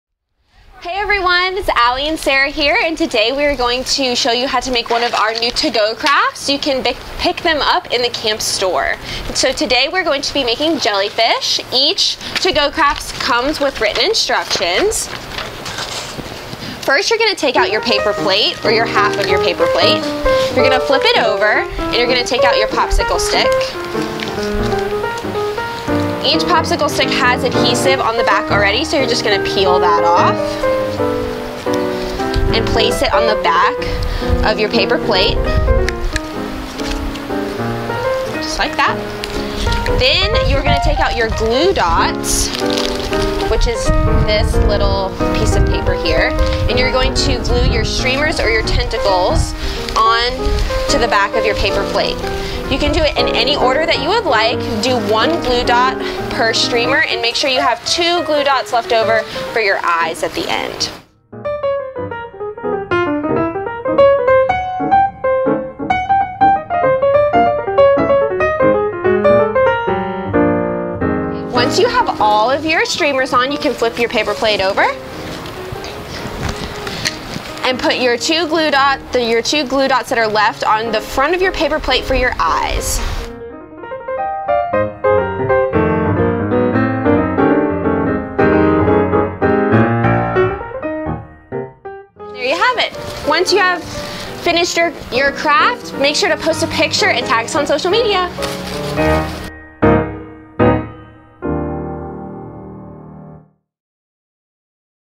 Jellyfish Craft Instructions
MBTP Rec Team members Allie and Sara show us how to make jellyfish with the free craft grab bags located in the MBTP General Store!